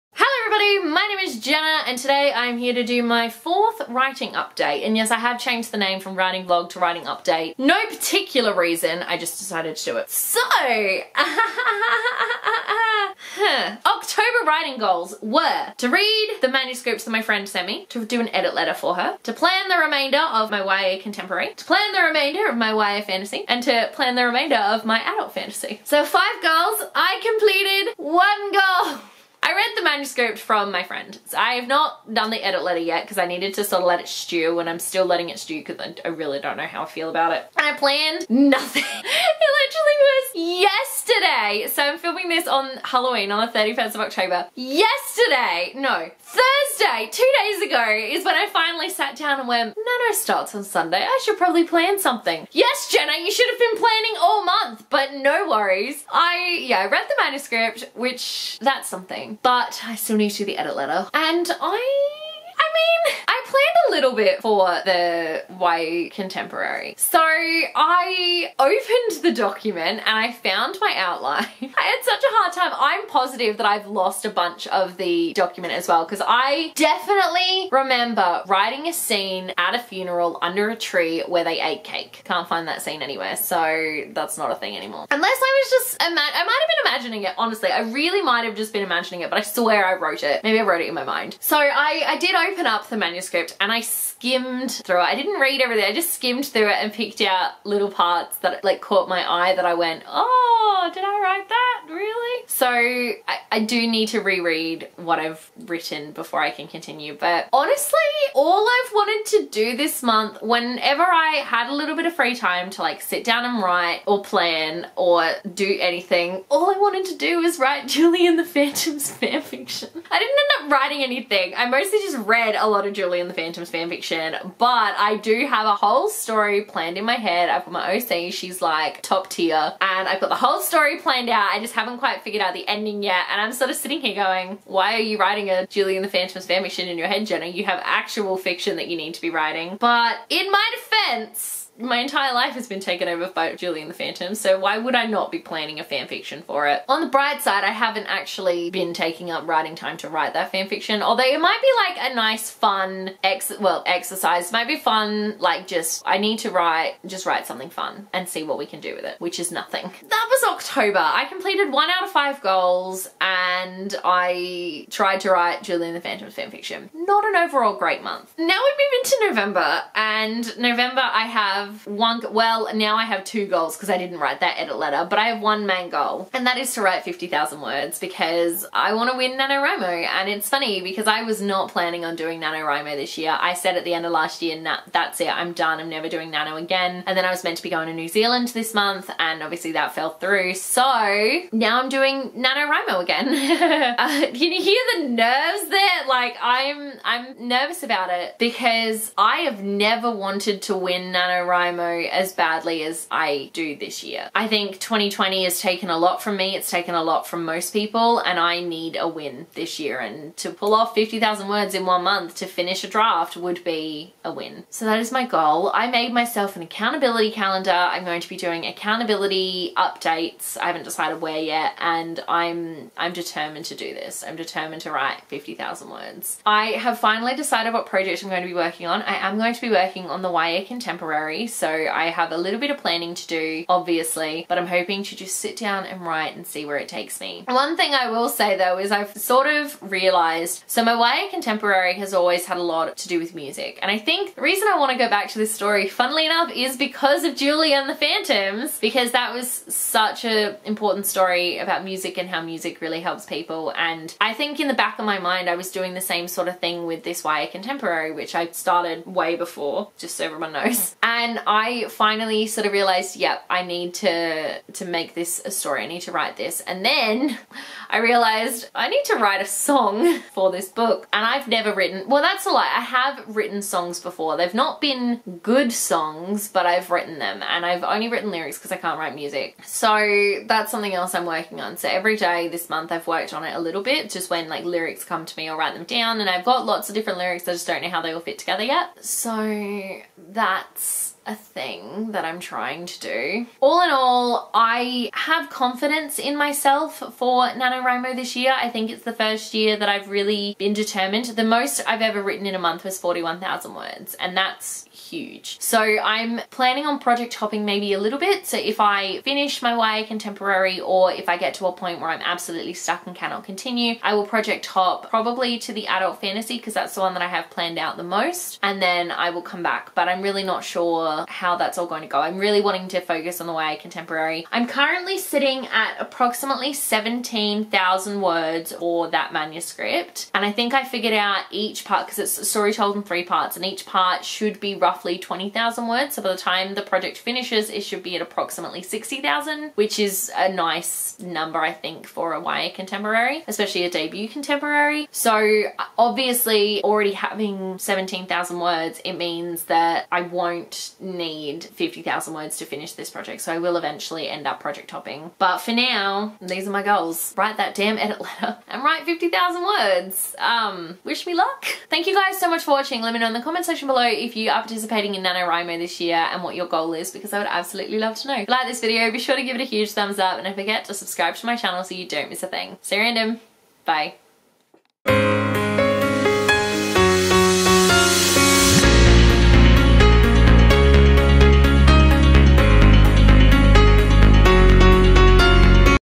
all i want to do is write julie and the phantoms fanfiction // Writing Update #4
Yes, all I want to do with my life is write Julie and the Phantoms fanfiction but honestly is anyone surprised?

Stay random!
~ Jenna

HIRE ME TO BE YOUR CRITIQUE READER!

GO BOOK SHOPPING!